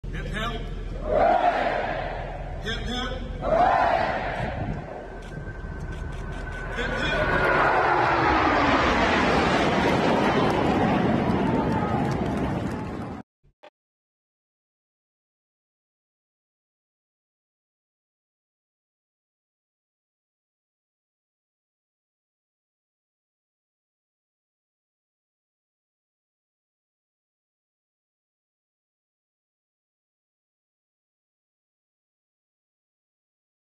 Blue Angels Fly Over Naval Academy as Grads Toss Caps in the Air
The Blue Angels flew over the graduation ceremony of the US Naval Academy in Annapolis, Maryland, on May 20, as the 2020 grads tossed their caps into the air.

Graduates were seen seated apart from each other during the ceremony, in line with COVID-19 social distancing precautions.